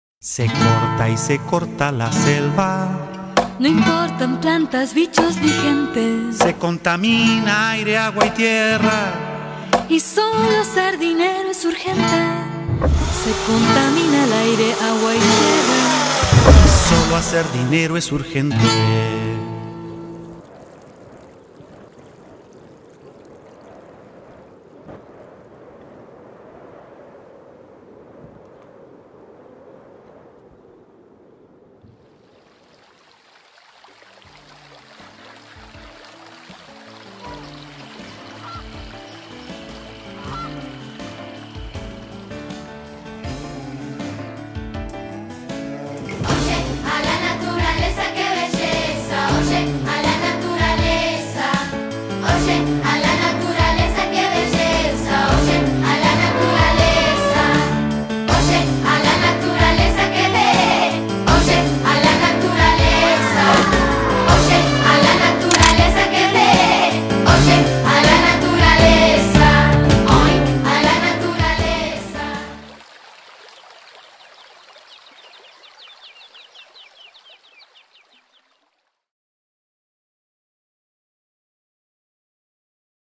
Oye a la naturaleza
Trabajo a concurso en el 2º Concurso Internacional de Cortos On Line por la cultura de la Sostenibilidad
Temática:Una nueva conciencia para la sostenibilidad ambiental
Resumen:El efecto del ser humano sobre la naturaleza ha sido devastador y destructivo, especialmente en el ultimo siglo. Necesitamos un cambio de conciencia que cambie explotación por sostenibilidad. Seguir haciendo oídos sordos a los problemas medioambientales dejará una herencia de sequía, contaminación y deforestacion a las generaciones futuras. El futuro del planeta esta unido al futuro del ser humano
Mensaje:Estar abiertos a un cambio de conciencia que nos permita escuchar a la naturaleza, hacernos responsables.